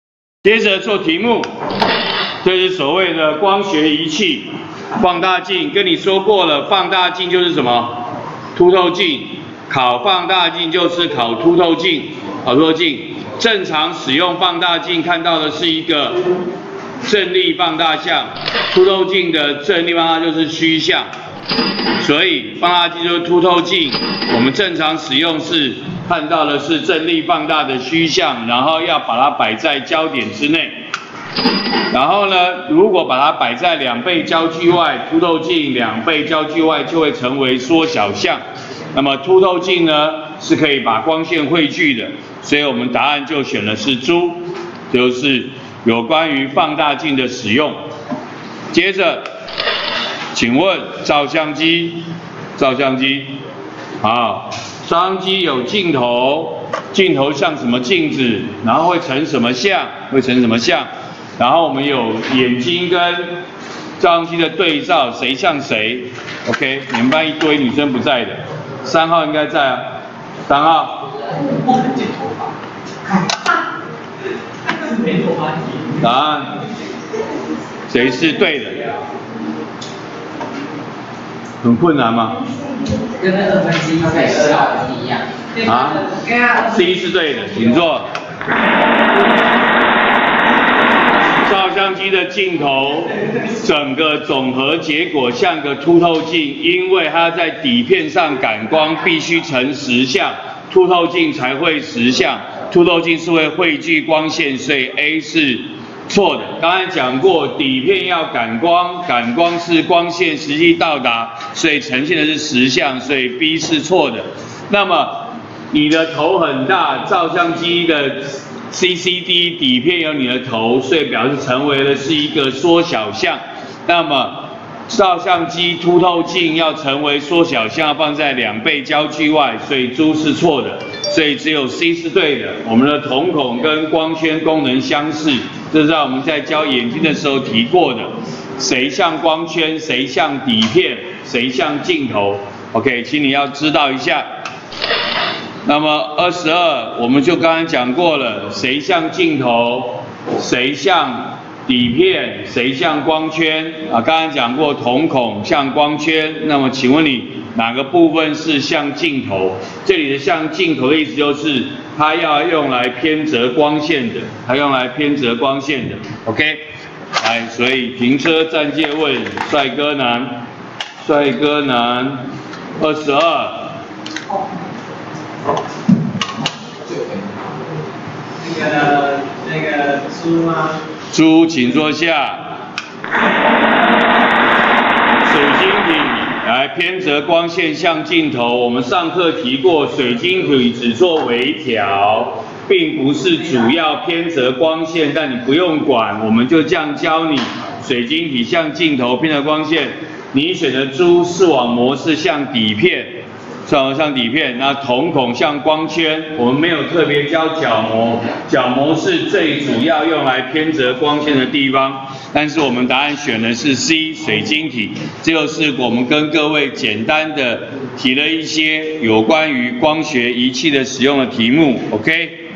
106學年複習光21：例題11；練習21、22
例題11問放大鏡使用問題。練習21問照相機問題。練習22問眼睛哪個構造類似照相機鏡頭。